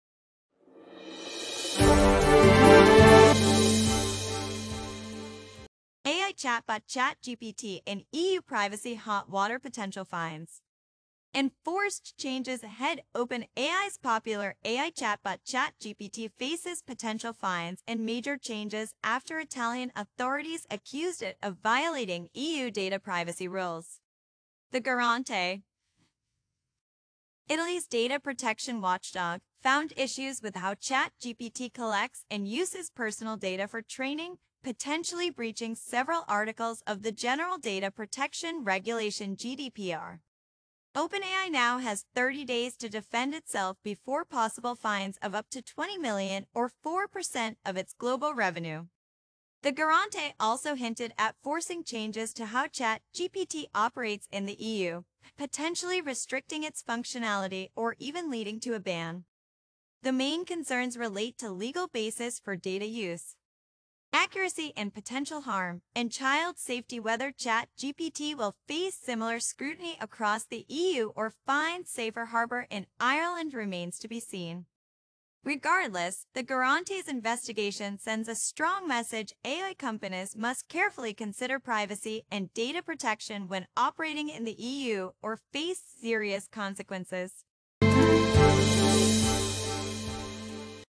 Is ChatGPT in deep trouble?
OpenAI's popular AI chatbot is facing potential EU fines and major changes after Italy accused it of violating data privacy rules.

The Garante, Italy's data watchdog, found issues with how ChatGPT collects and uses personal data for training, potentially breaching the General Data Protection Regulation (GDPR).

OpenAI has 30 days to defend itself against €20 million fines or 4% of global revenue! The Garante also hinted at restricting ChatGPT's functionality or even banning it in the EU.

Key concerns:

Legal basis for data use: OpenAI's claim might not hold up under GDPR.
Accuracy and potential harm: ChatGPT's "hallucinations" raise concerns.
Child safety: Additional restrictions could be imposed.

Will ChatGPT face similar scrutiny across the EU?  Can it find a safe haven in Ireland? 🇮🇪 This case sends a strong message: AI companies must prioritize data privacy in the EU, or face consequences!

Watch this video to learn more about:

* The details of the ChatGPT data breach investigation
* The potential impact on OpenAI and other AI companies
* The future of AI privacy regulations in the EU

ChatGPT, AI, privacy, GDPR, data breach, EU, Italy, fines, regulations, OpenAI, AI ethics, legal, technology, future, artificial intelligence, chatbot, news, Europe, cybersecurity,